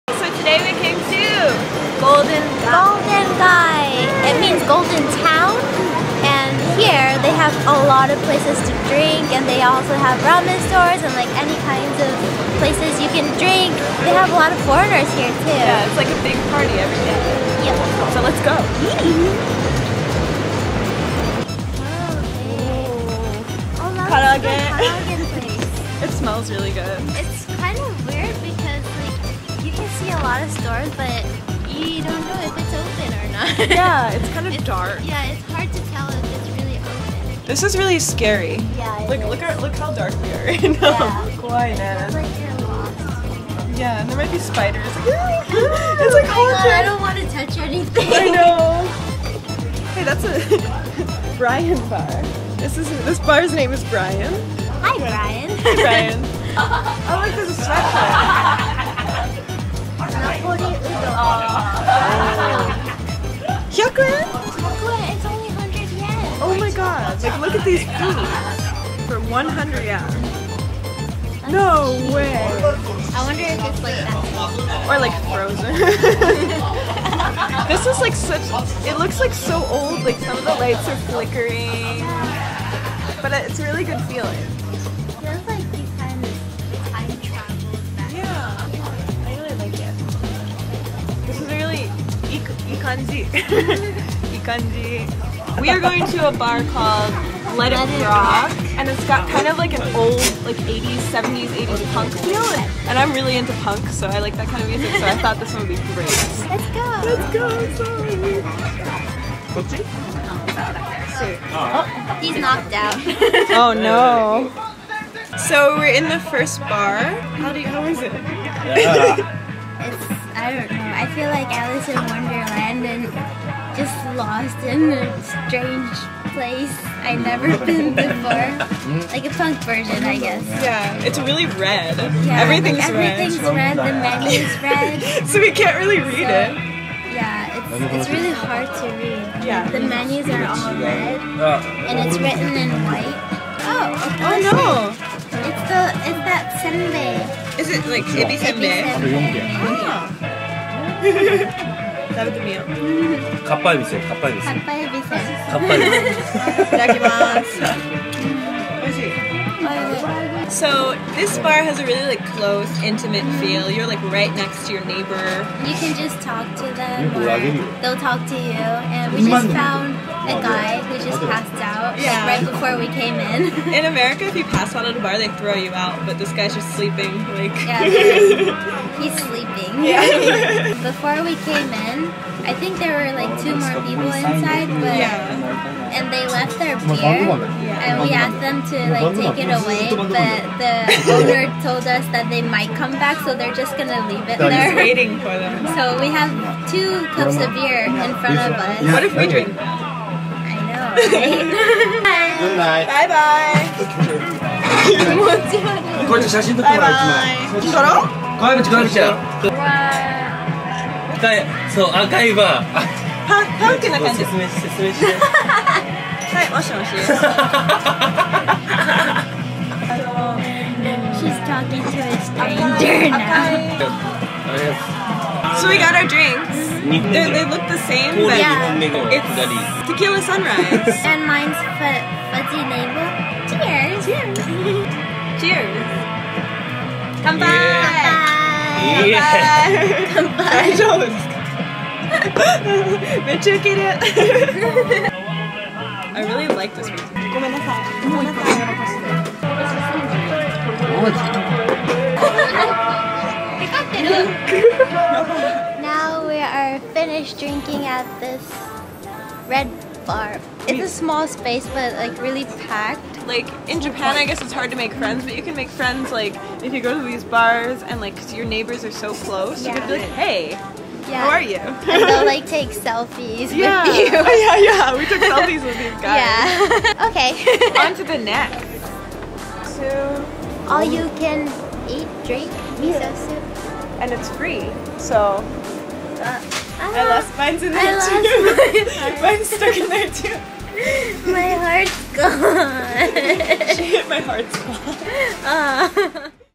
Tokyo's Hottest Drinking Spot 東京で１番イケてる外人にオススメのバー街
We went to Golden Gai which is the most popular drinking place for foreigners.

外国人に超人気の吞みスポット
ゴールデン街にいきました〜

★Please check out Facebook and twitter!
私たちのFacebook,twitterもチェック！
日常をUPしてます！